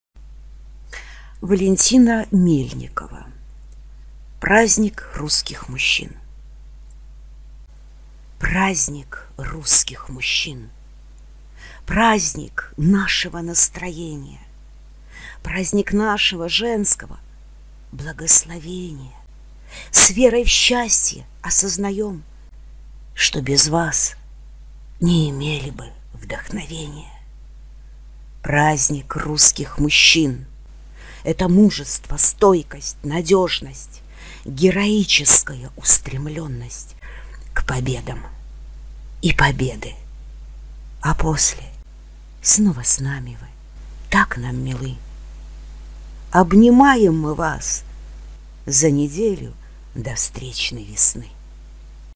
Праздник русских мужчин
Видео сделано Ольгой Грибановой. Спасибо! Стихи Валентины Мельниковой